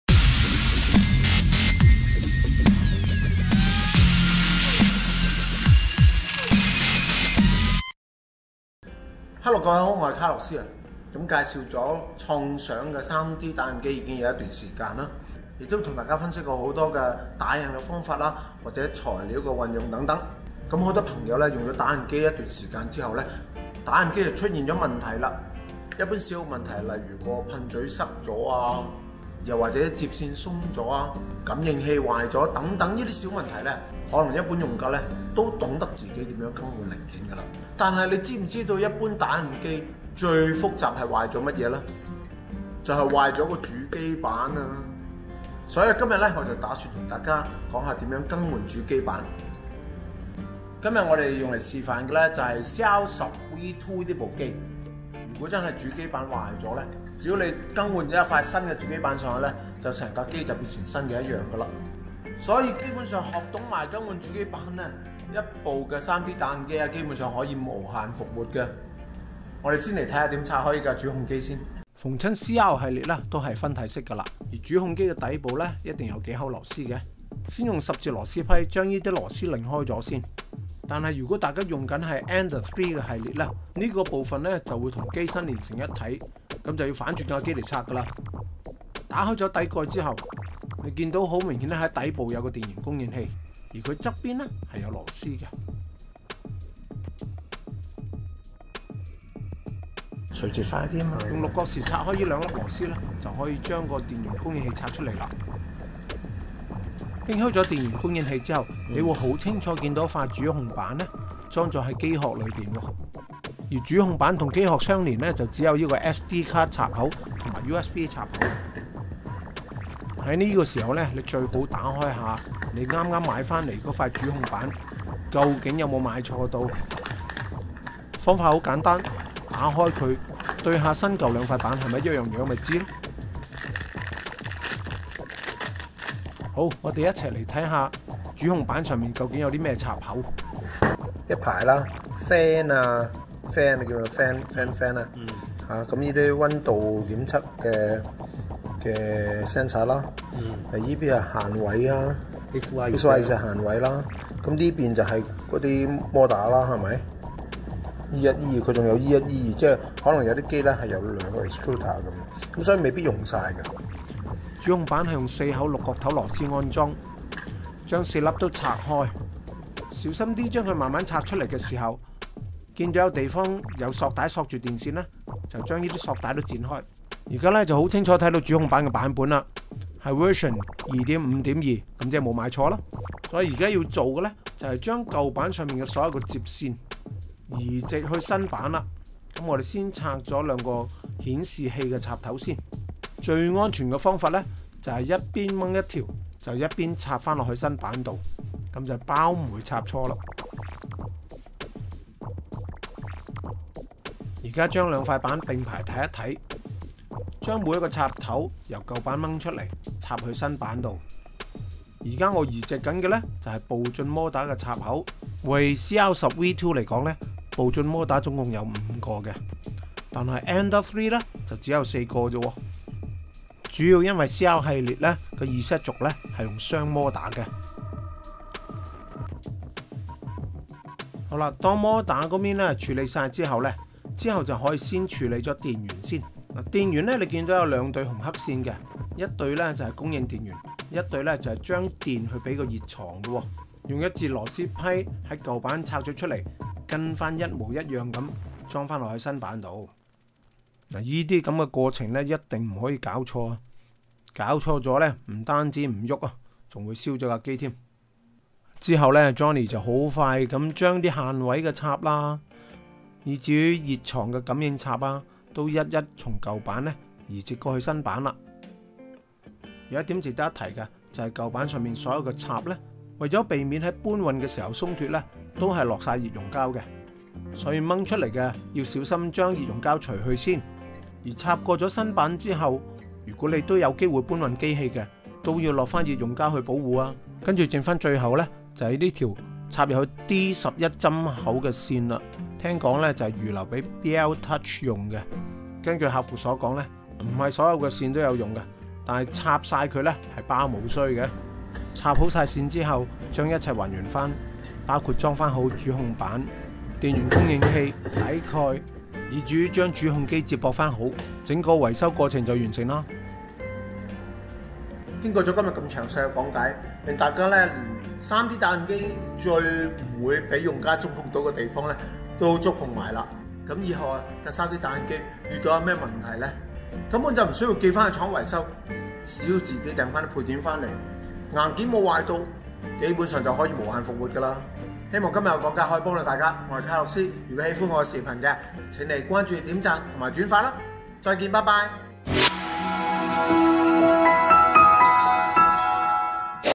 【創想系列】更換打印機主控板
3D打印機其實和一部電腦無異，都是由主機板和輸入輸出組成，壞了主板和砌電腦一樣，更換主板便可重新運作，一定要學起來！